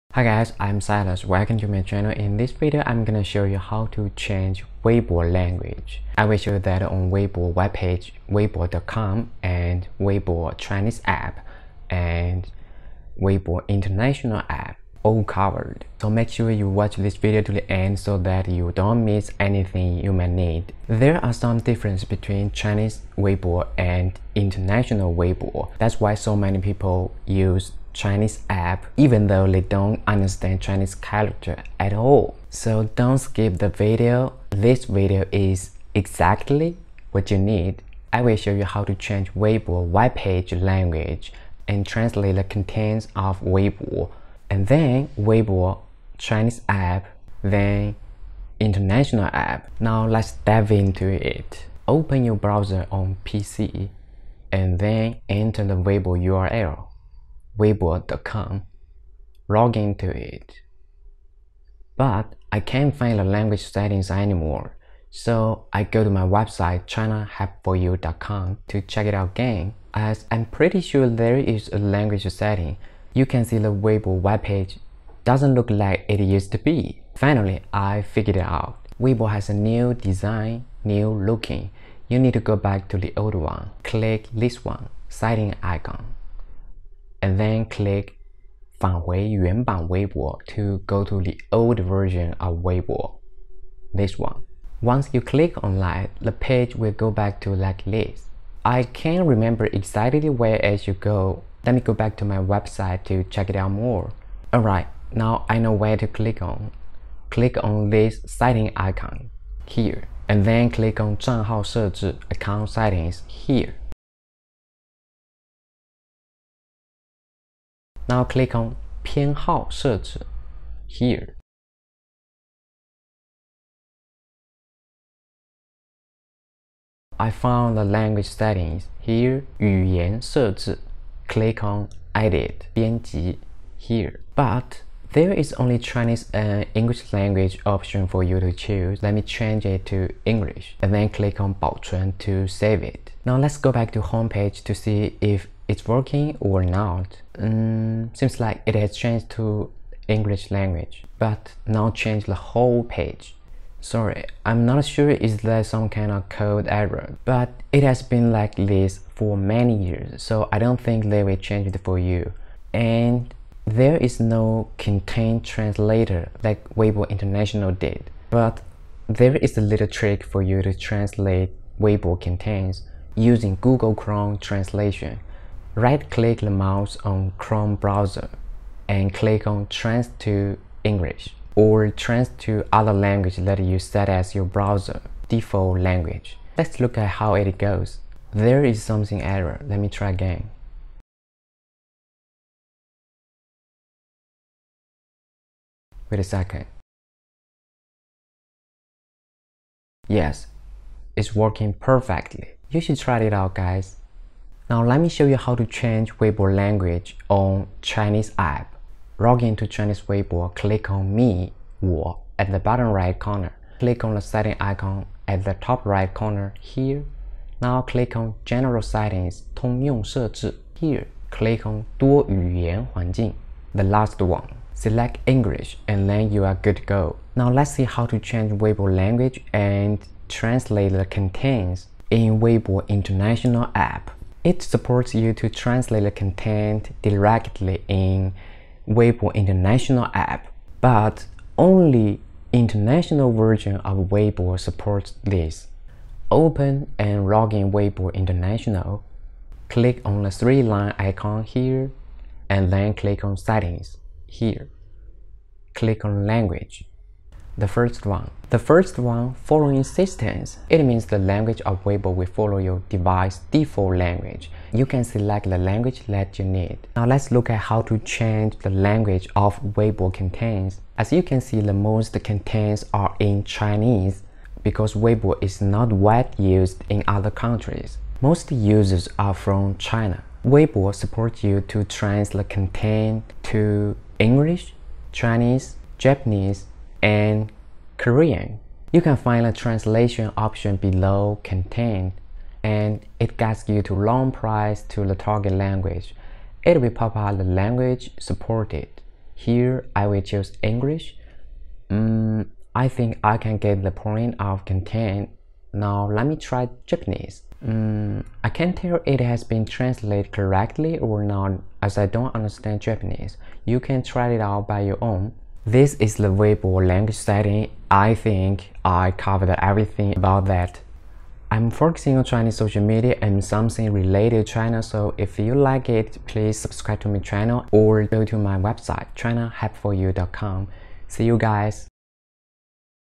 Weibo (微博)language setting And Weibo translation | Weibo English | Weibo translator
Is there any Weibo English or any tools for Weibo translation❓

🔈Yes, Weibo supports Chinese and English language for you to use. And Weibo supports you to translate the contents to ✅Chinese, ✅English, ✅Japanese and ✅Korean directly.

If you can't read Chinese characters (Chinese language) then, this video is exactly what you need, make sure watch this video to the end so that you won't miss any important info you might need.